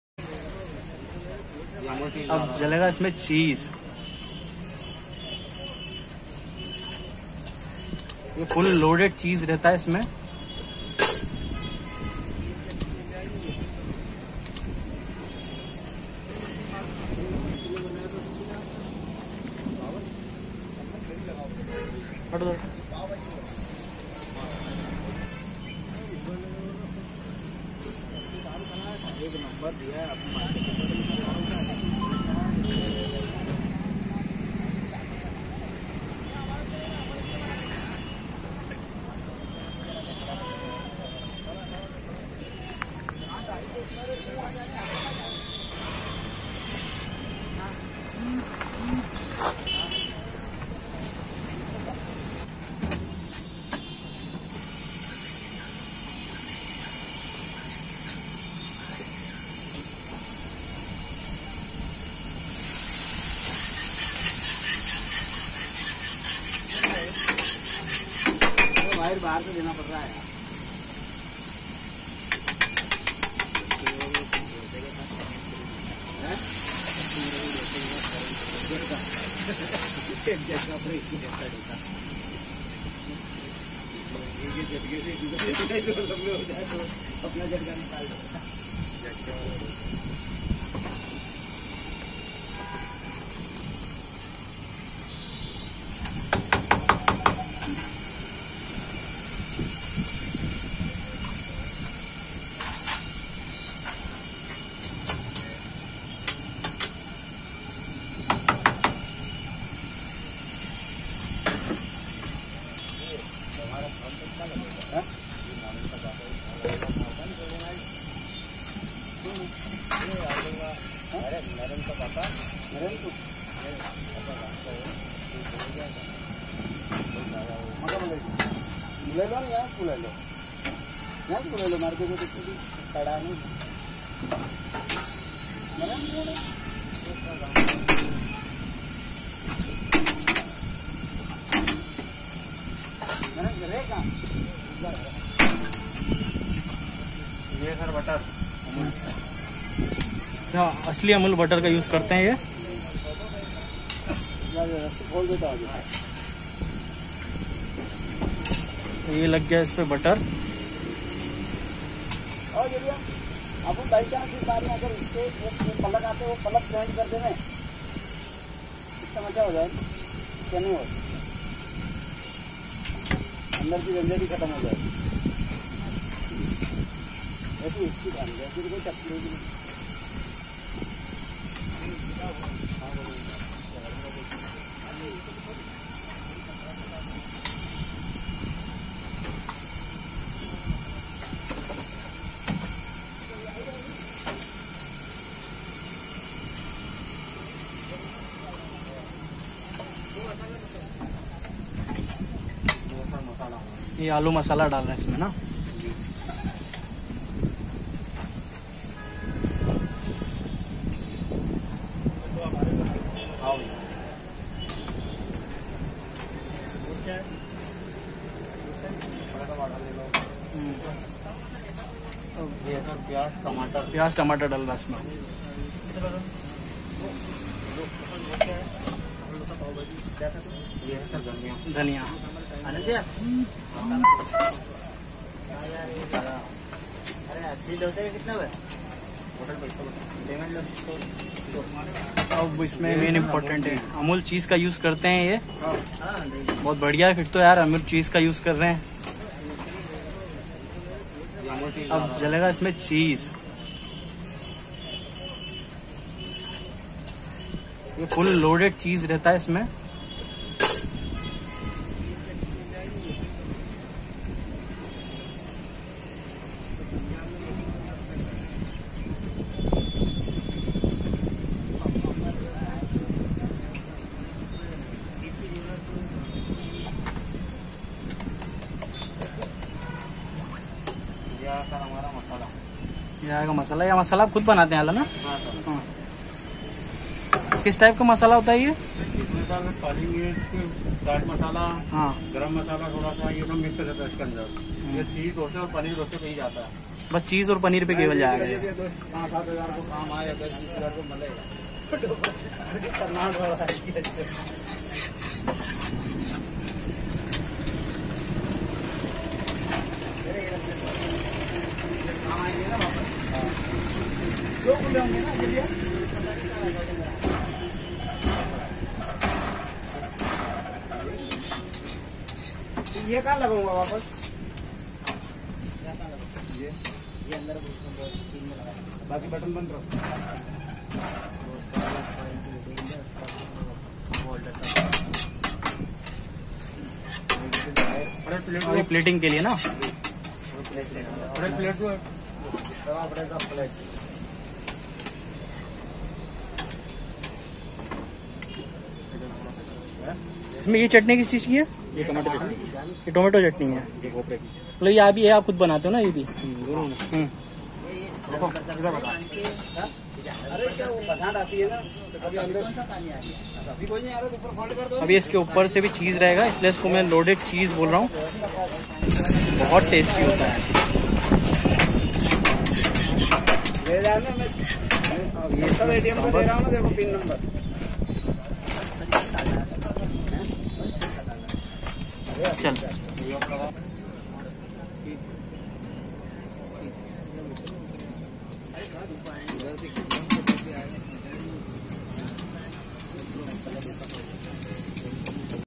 Cheese Loaded Dosa | Street Style Cheese Loaded Dosa | Cheesy Dosa | Indian Street Food Vlog
Location :
Aanand Dosa
Scheme No .140 , Pipliyana Square , Indore

Food Description : Street Style Loaded Cheese Dosa with Great Cheesiness, Texture and Taste.
If you Come here , Must Try Food.

Shooting & Editing - Satyam Mishra
Producer & Direction - Shivam Mishra